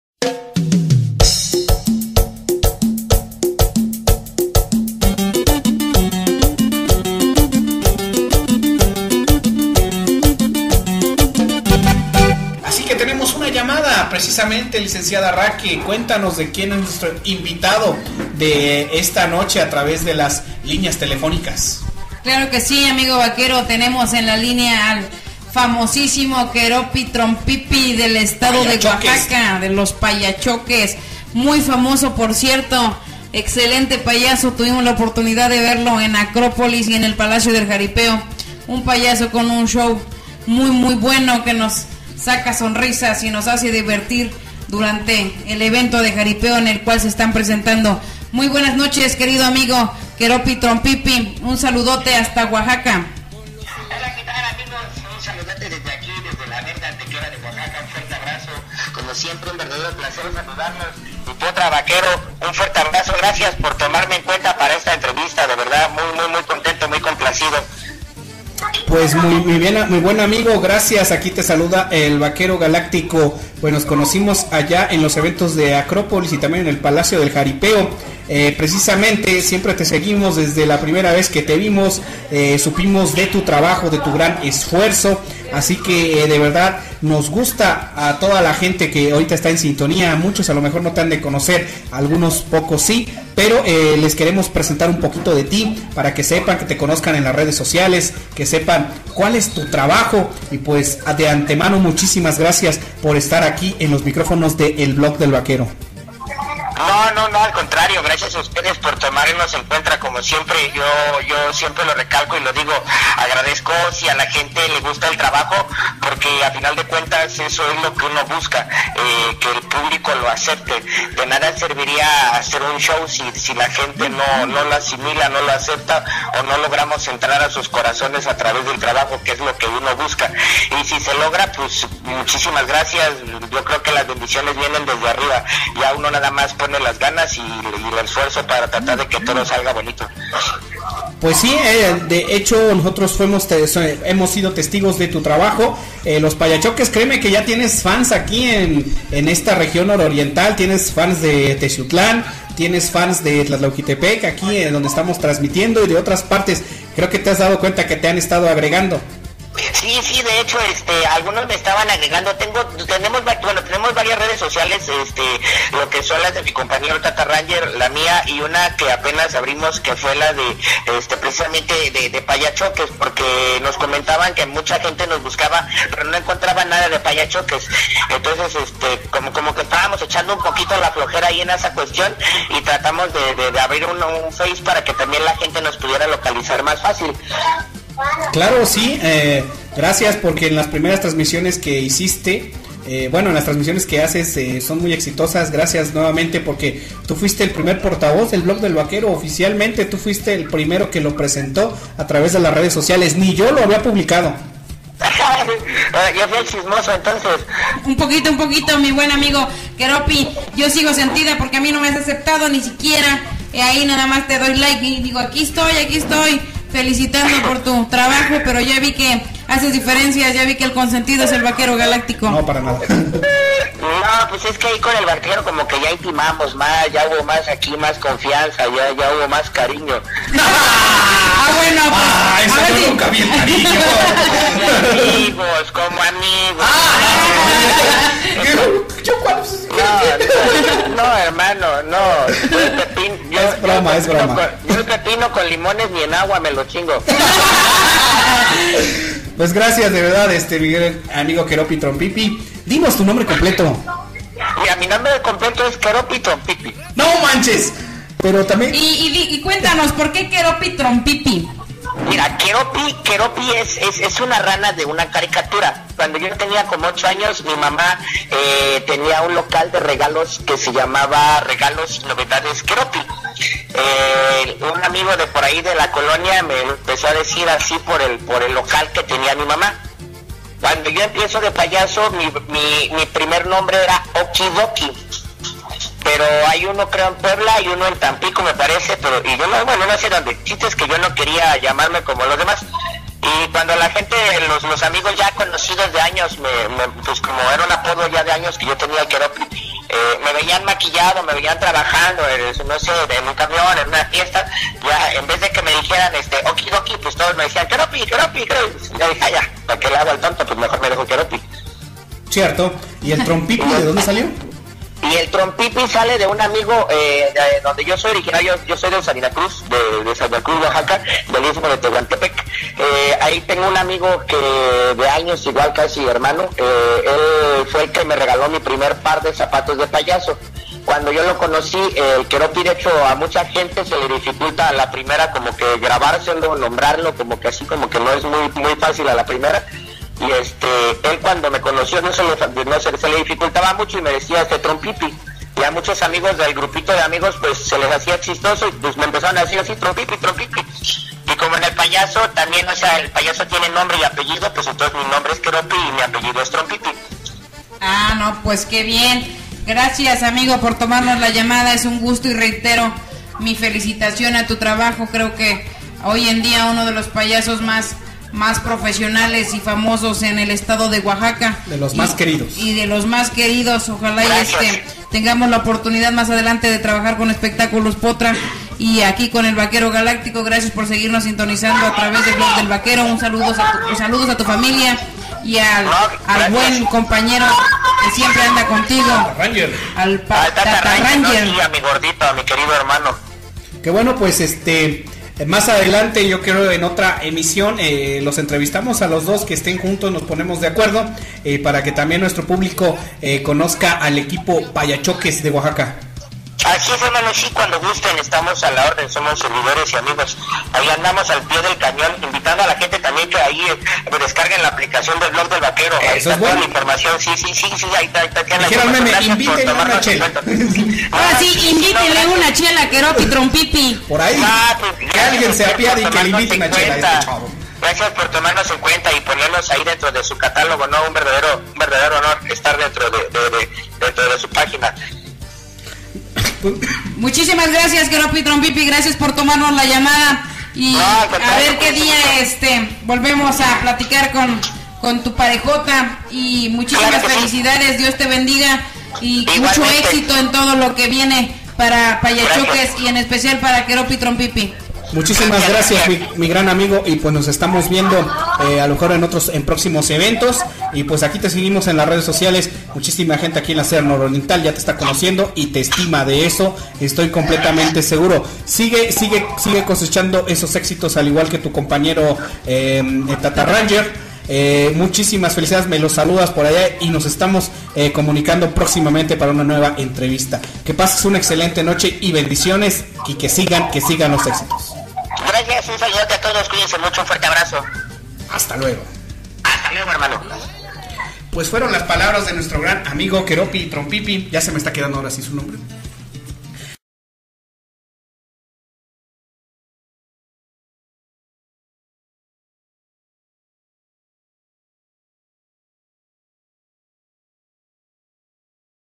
ENTREVISTA AL PAYASO KEROPY TROMPIPI DE LOS PAYACHOQUES OAXACA
Keropy Trompipi Entrevista a través de los Micrófonos de El Blog Del Vaquero en el programa en vivo de elsondelvaquero. Payachoques Oaxaca Acción.